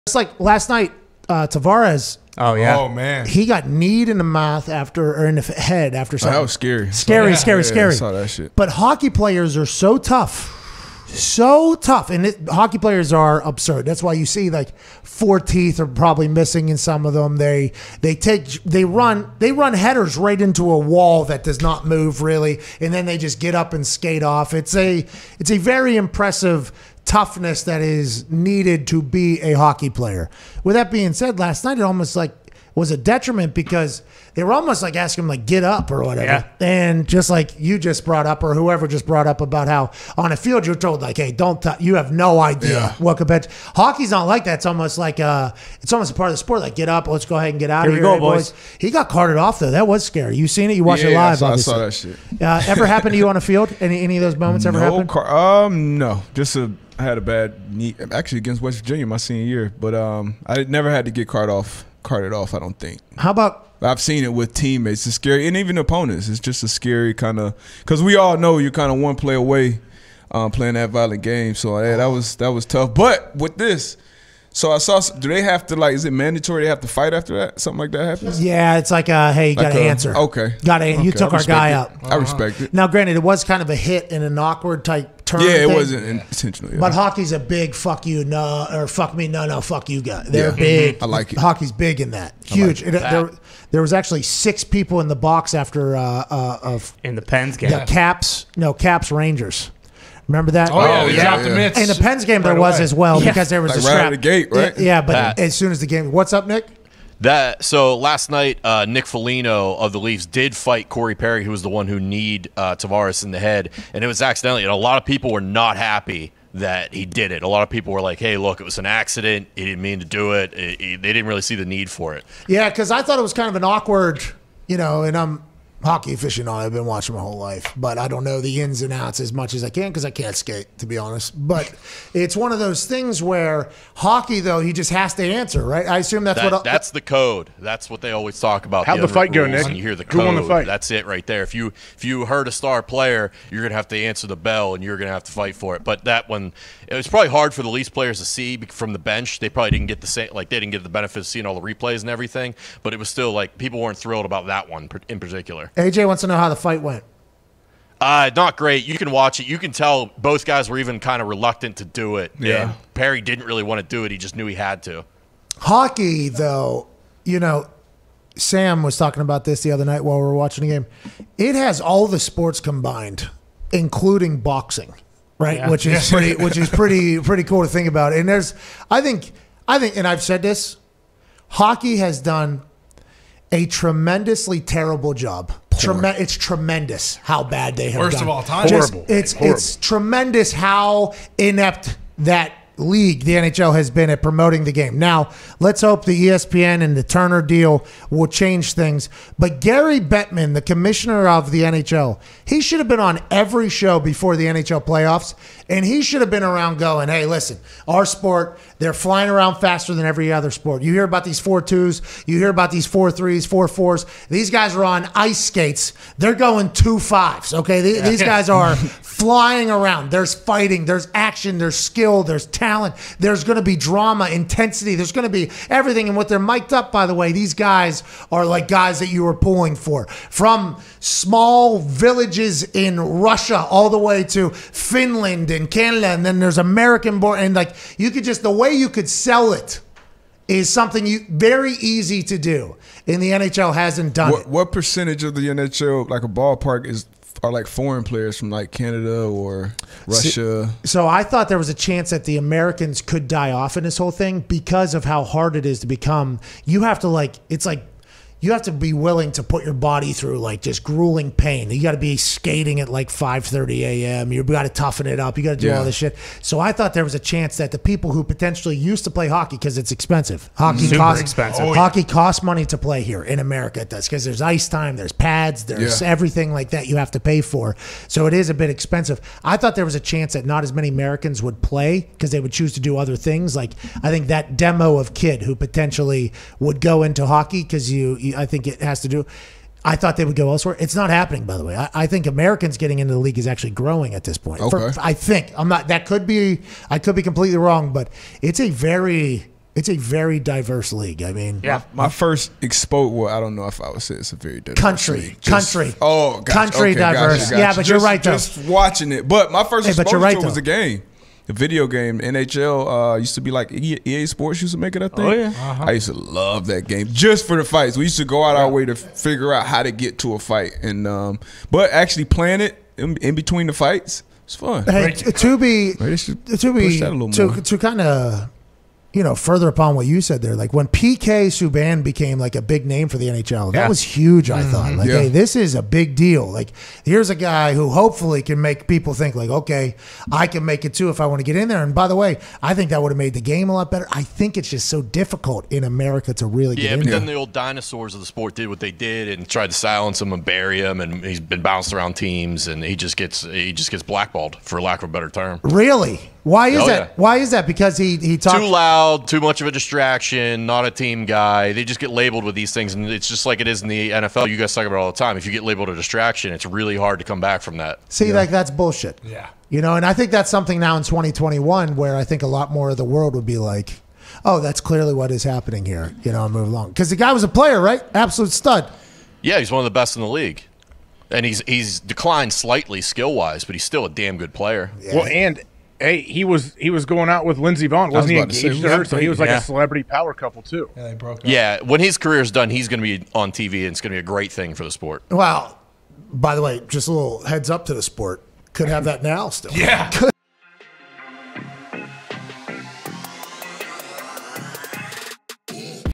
Pat McAfee Reacts To John Tavares Scary Injury In NHL Playoff Game
We are happy to hear that Tavares is doing alright... this was a pretty scary moment.